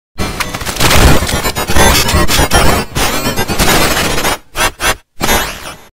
(NEW EFFECT) Klasky Csupo in G-Major 7236235782
How to make G-Major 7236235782:

Video: ChordedCityNight + G-Major 74

Pitches:
-89
-81
-78
-76
-75
-71
-68
-67
-63
-43
-23
-13
-10
-6
0
+2
+3
+12
+16
+26
+36
+46
+56